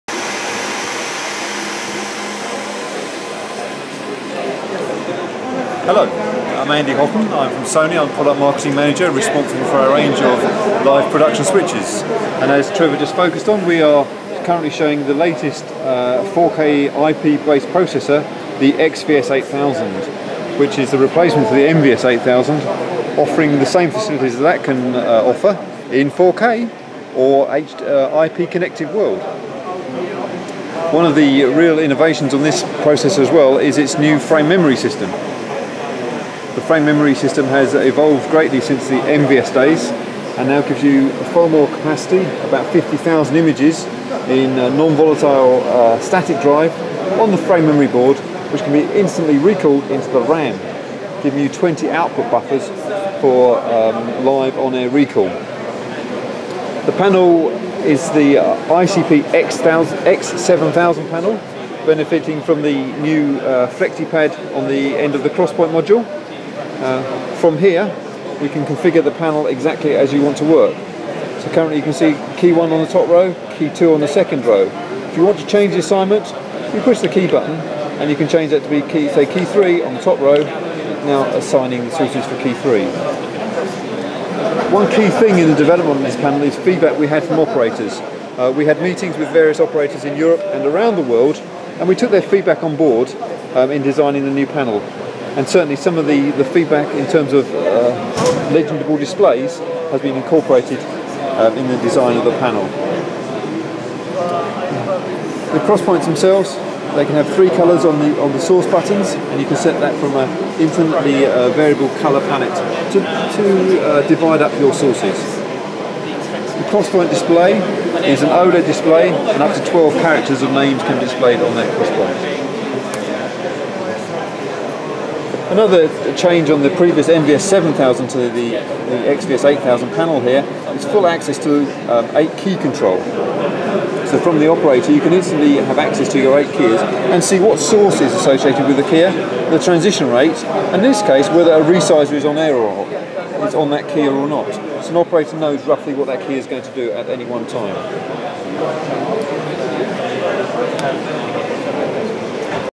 New XVS 8000 Processor & Panel from Sony
XVS 8000 Processor & Panel from Sony at The Vision Mixers Show 2016